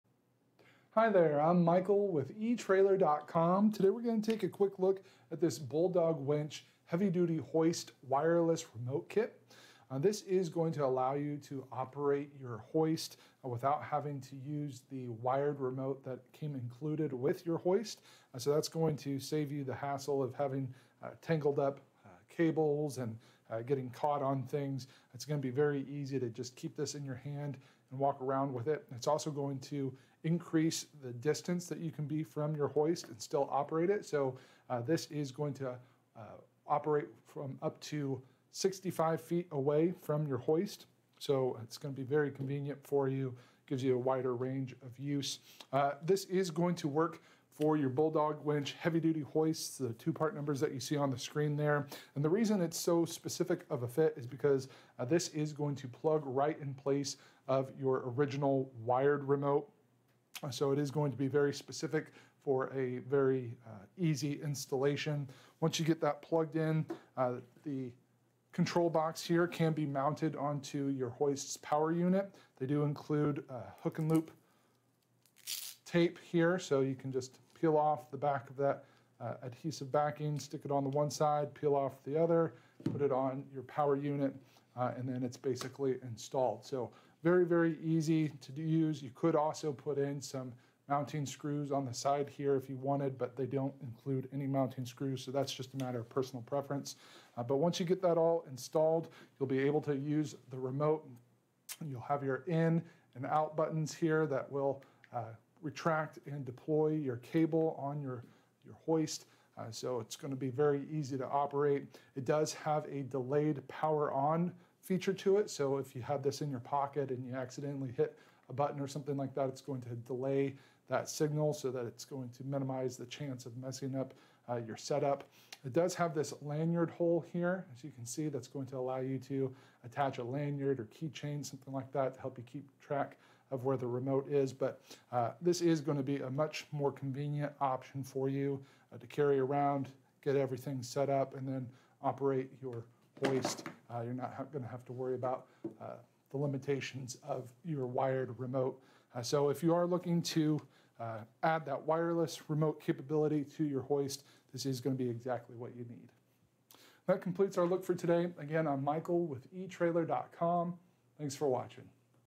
etrailer | Review of Bulldog Winch Heavy-Duty Hoist Wireless Remote Kit - BDW82SB
Hi there. I'm Michael with etrailer.com. Today, we're gonna take a quick look at this Bulldog wench heavy-duty hoist wireless remote kit. This is going to allow you to operate your hoist without having to use the wired remote that came included with your hoist. So that's going to save you the hassle of having tangled up cables and getting caught on things. It's gonna be very easy to just keep this in your hand and walk around with it.

It's also going to increase the distance that you can be from your hoist and still operate it. So this is going to operate from up to 65 feet away from your hoist. So it's gonna be very convenient for you. It gives you a wider range of use. This is going to work for your Bulldog wench heavy-duty hoist.

The two part numbers that you see on the screen there. And the reason it's so specific of a fit is because this is going to plug right in place of your original wired remote. So it is going to be very specific for a very easy installation. Once you get that plugged in, the control box here can be mounted onto your hoist power unit. They do include hook-and-loop tape here.

So you can just peel off the back of that adhesive backing, stick it on the one side, peel off the other, put it on your power unit, and then it's basically installed. So very, very easy to be used. You could also put in some mounting screws on the side here if you wanted, but they don't include any mounting screws. So that's just a matter of personal preference. But once you get that all installed, you'll be able to use the remote and you'll have your in and out buttons here that will retract and deploy your cable on your hoist.

So it's gonna be very easy to operate. It does have a delayed power on feature to it. So if you have this in your pocket and you accidentally hit a button or something like that it's going to delay that signal so that it's going to minimize the chance of messing up your setup. It does have this lanyard hole here, as you can see, that's going to allow you to attach a lanyard or keep chain, something like that, to help you keep track of where the remote is. But this is gonna be a much more convenient option for you to carry around, get everything set up, and then operate your hoist. You're not gonna have to worry about the limitations of your wired remote. So if you are looking to add that wireless remote capability to your hoist, this is gonna be exactly what you need. That completes our look for today.